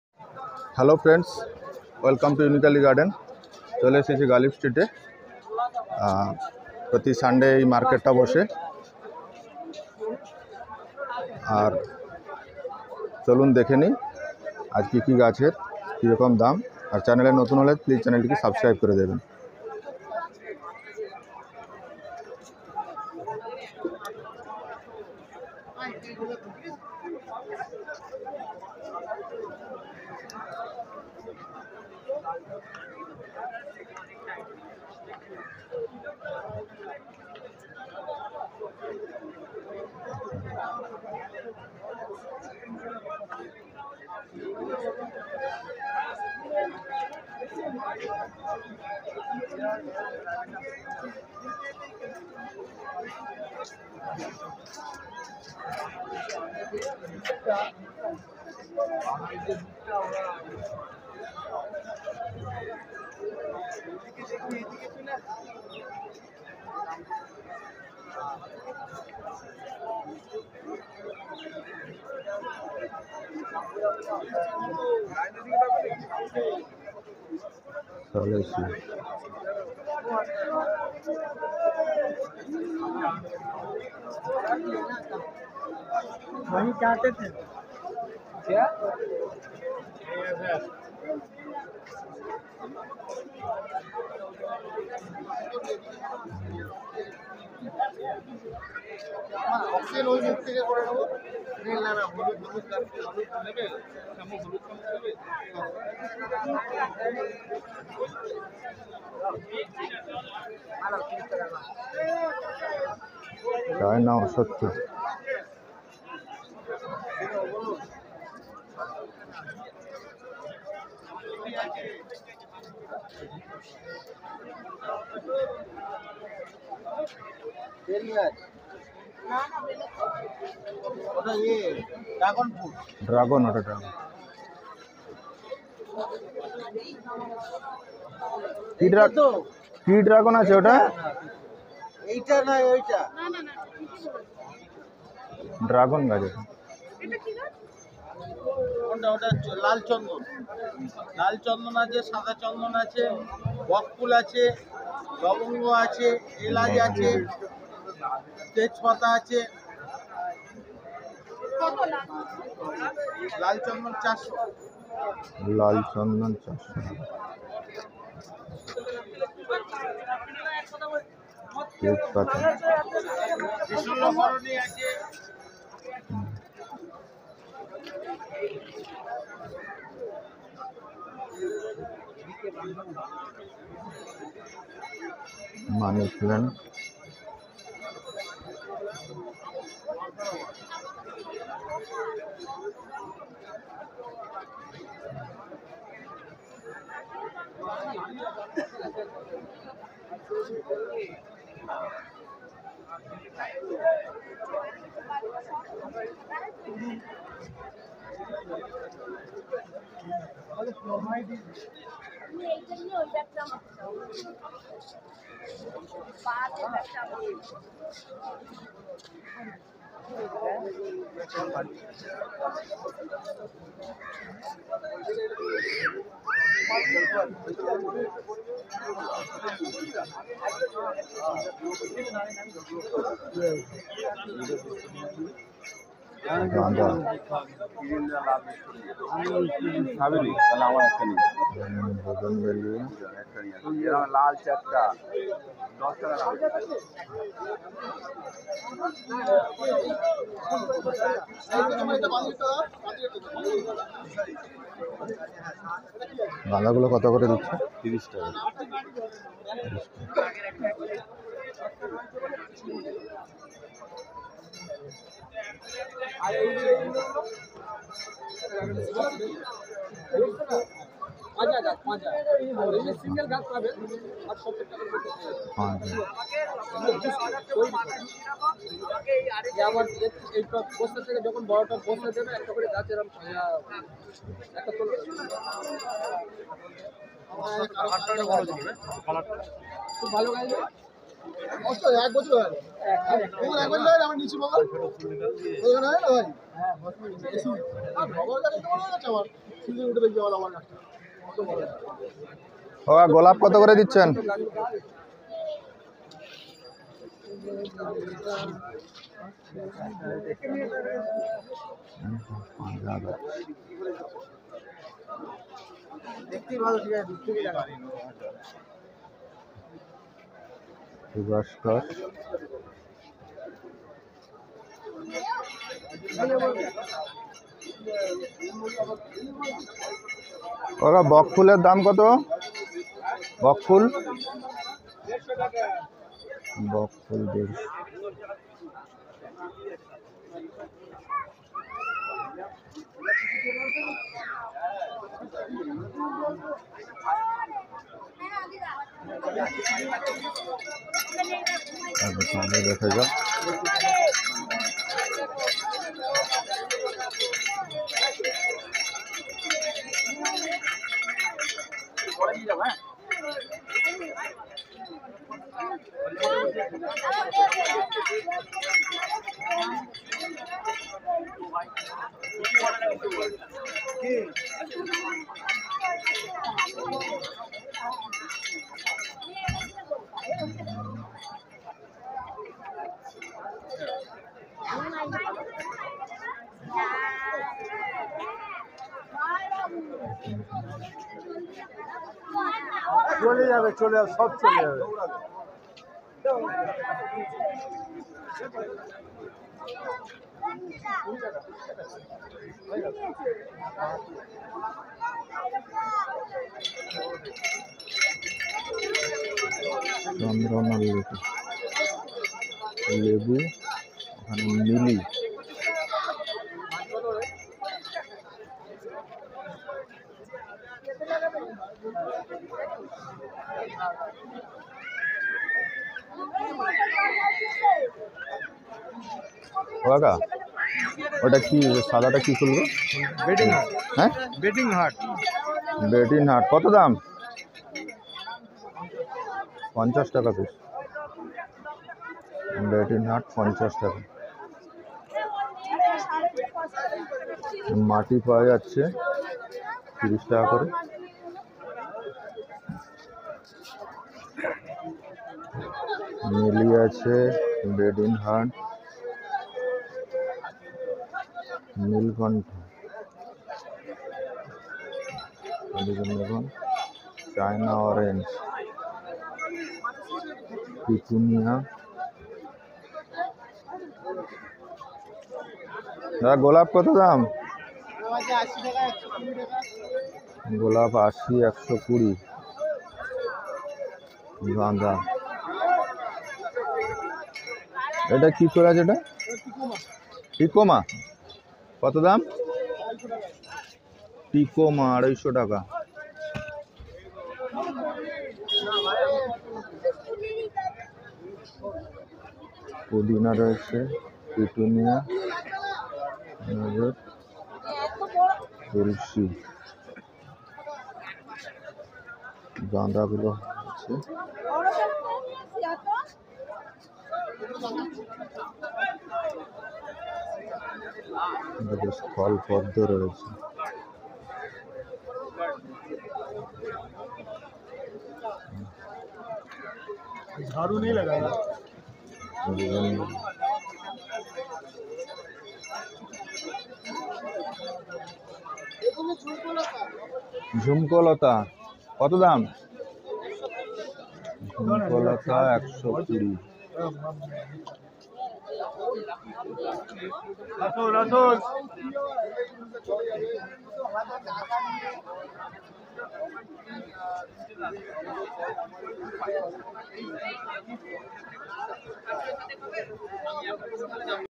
Best winter season flower market in Gallif street.Shyambazar, kolkata. Uniquely garden.
সমস্ত ধরনের গাছের সম্ভার এখানে. অন লাইনে ও গাছ কিনতে পারবেন এখানে.

সব থেকে ভালো হয় আপনি এখানে এসে গাছ দেখে কিনে নিয়ে যান.

এই মার্কেটে কিভাবে আসবেন তা আমার ভিডিও তে বলে দেওয়া আছে। আশাকরি কোনো অসুবিধা হবে না.

         ধন্যবাদ